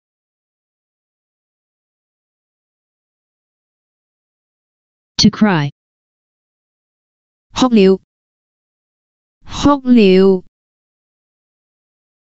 "TO CRY" in Cantonese (哭了) - Flashcard
[huk1 liu5] "TO CRY" in Cantonese (哭了) - Flashcard. Additional information about the word: to cry.